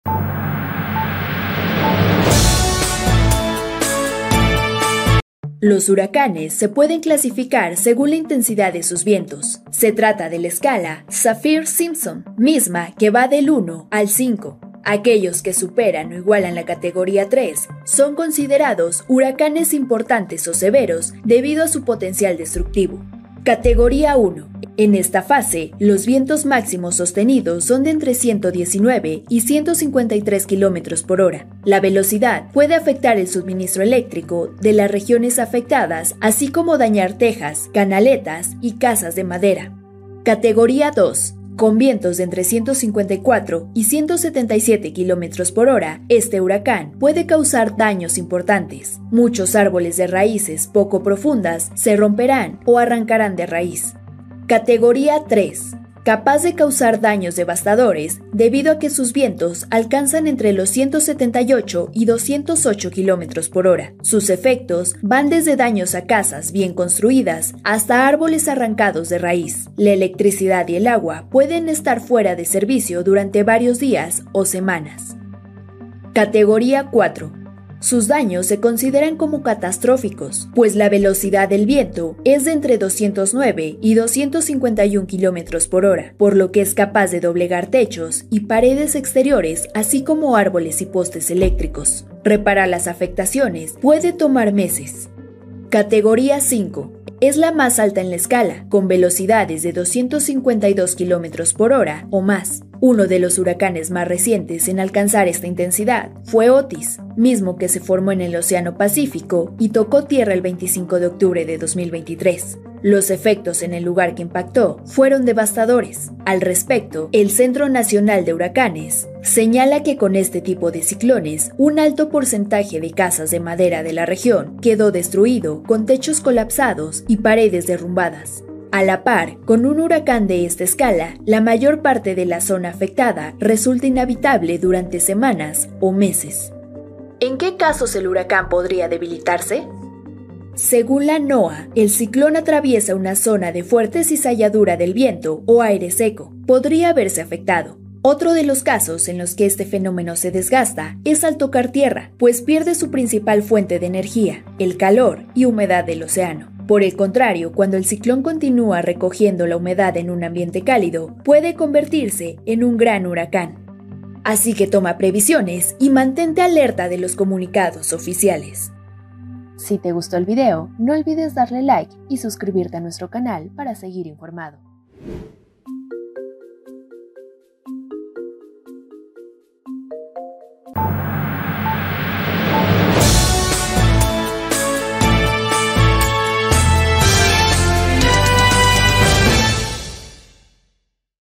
***** BERYL YA ES CATEGORÍA 5 ***** CATEGORÍAS SEGÚN LA INTENSIDAD DEL HURACÁN
CATEGORÍAS SEGÚN LA INTENSIDAD DEL HURACÁN
Sergio Molina | NOTICIAS